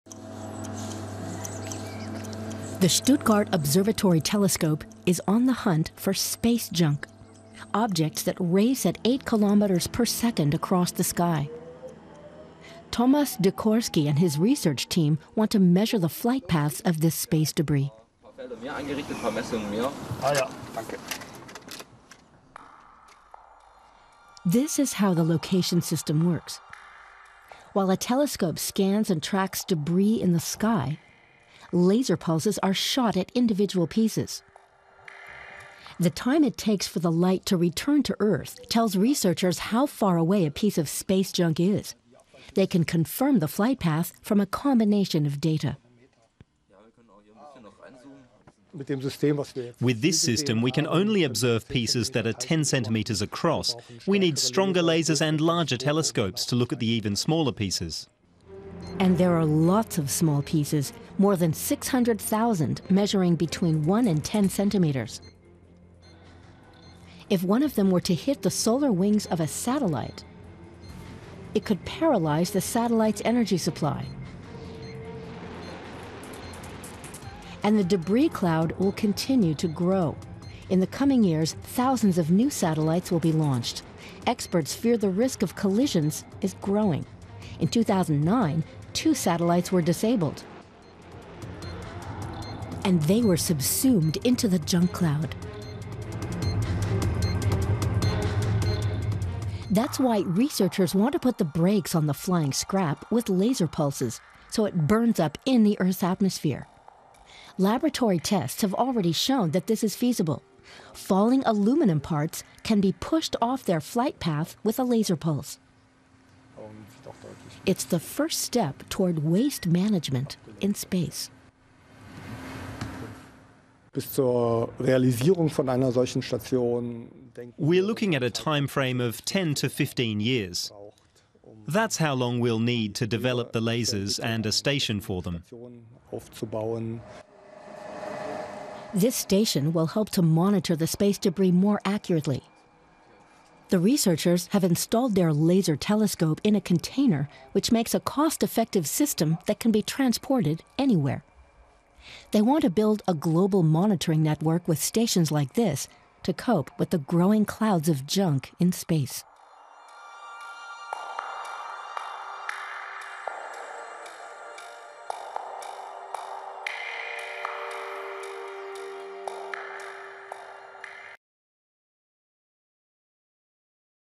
Cosmic cleaning: junk in orbit | DW English
Space exploration has forced scientists to confront a new challenge: cleaning up the waste left in its wake. Objects like satellites don’t last forever. Once defunct, they continue to orbit in space, where they pose a risk of collision.